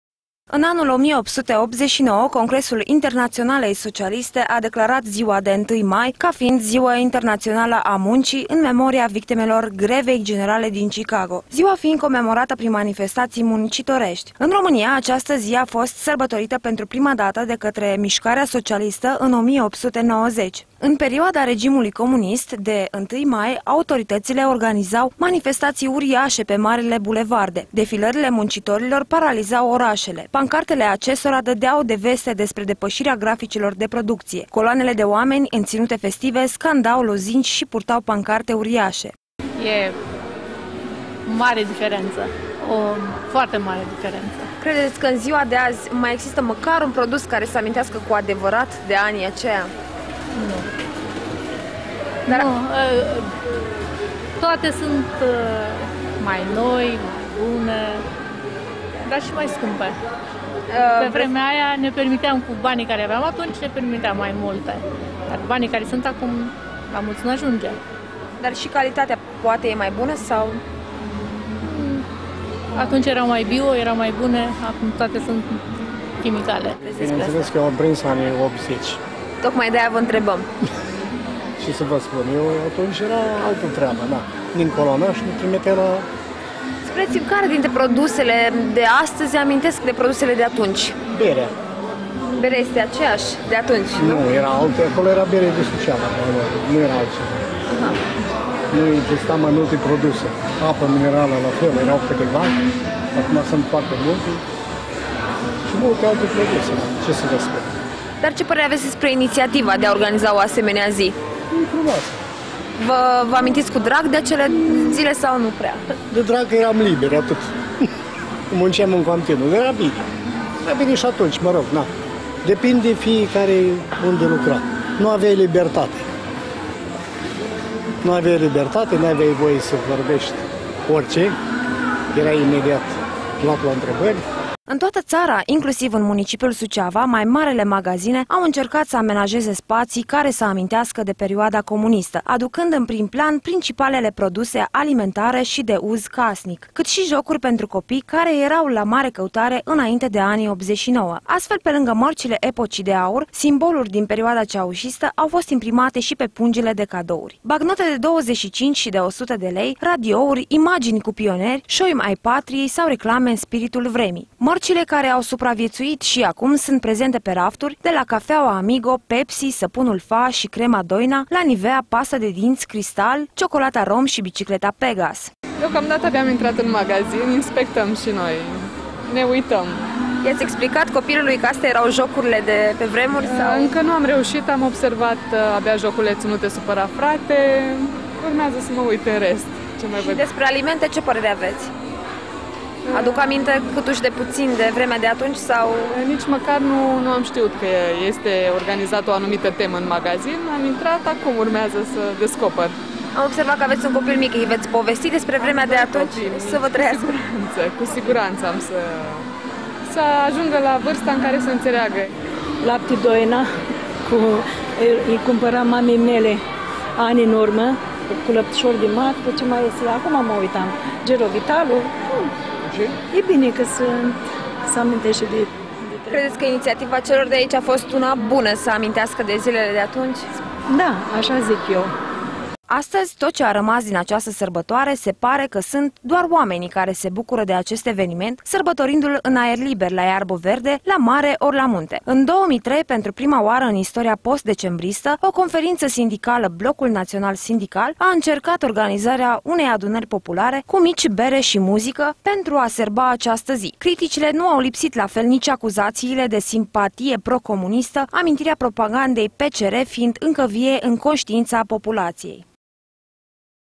Amintiri din comunism
Daca vreți sa retrăiți câteva dintre frânturi de tinerețe comunistă, puteți degusta și aprecia produse care se fabricau în acea vreme. Mica expoziție a fost realizată chiar de 1 mai într-un centru comercial din Suceava. De remarcat este că toate produsele sunt expuse pentru vânzare .